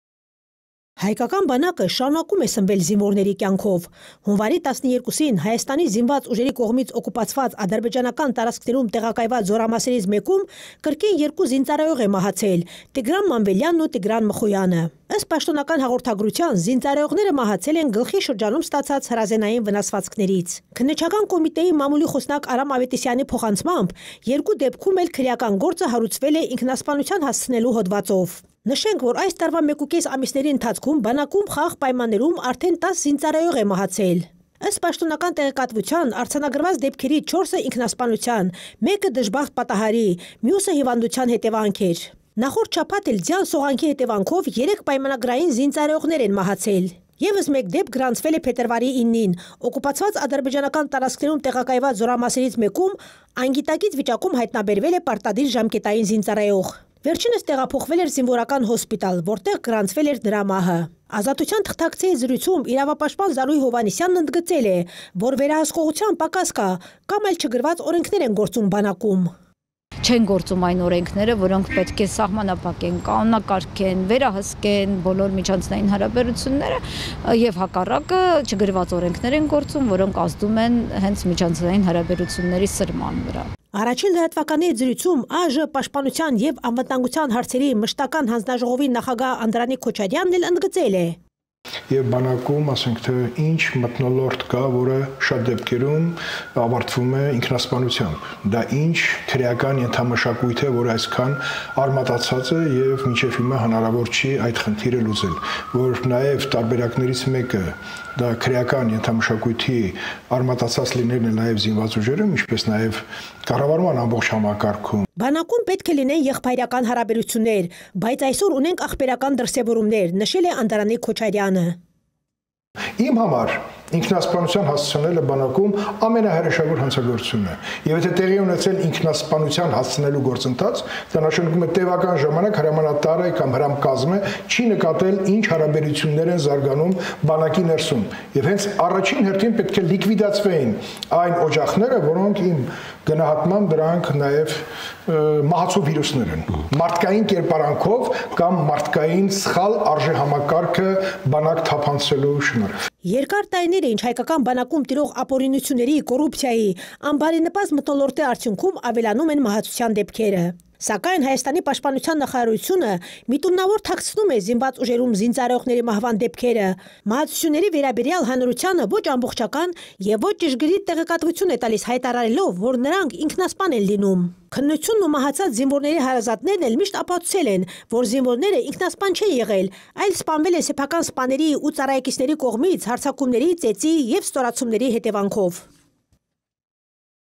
Հայկական բանակը շարունակում է սնվել զինվորների կյանքով
Հունվարի 12-ի Հայաստանի զինված ուժերի կողմից օկուպացված ադրբեջանական տարածքներում տեղակայված զորամասերից  մեկում կրկին երկու զինծառայող է  մահացել՝ Տիգրան Մանվելյանն ու  Տիգրան  Մխոյանը: